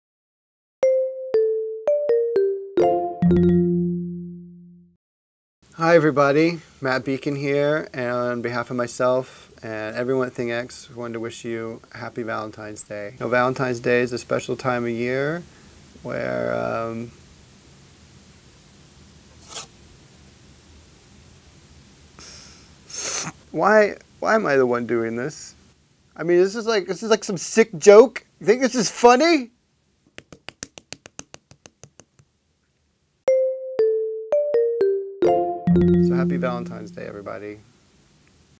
XHow - Keep the flame alive (a special Valentine's message)
Matt Beacon sends you a message of love for Valentine's Day.